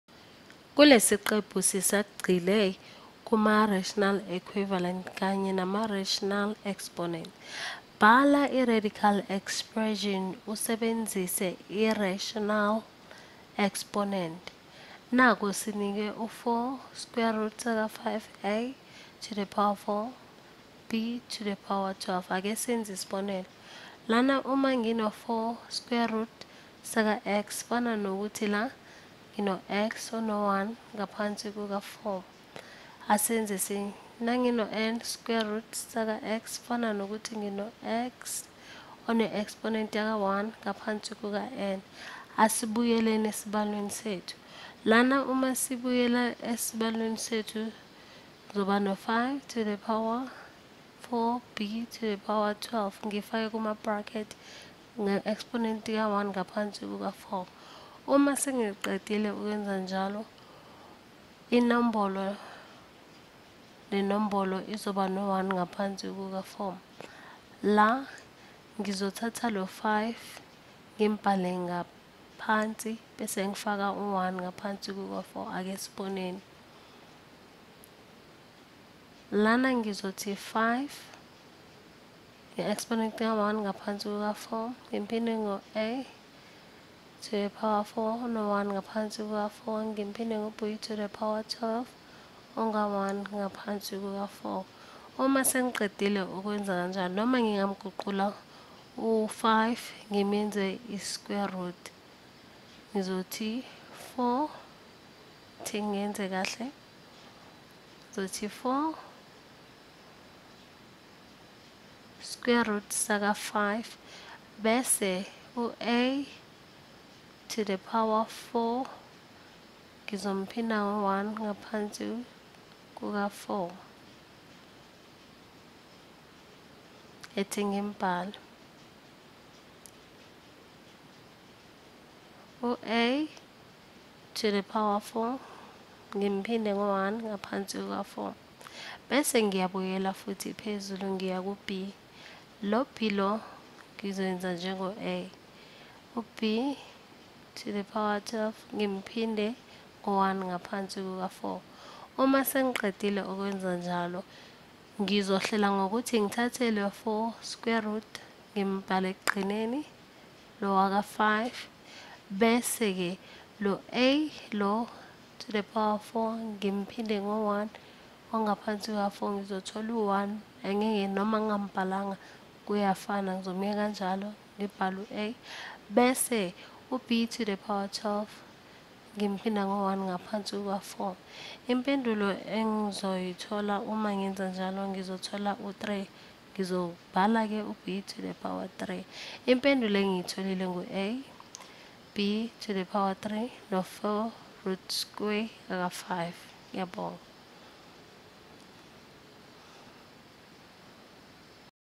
Radical Equivalent to Rational Exponents 2
This original Khan Academy video was translated into isiZulu by Sibongile Mavundla. The translation project was made possible by ClickMaths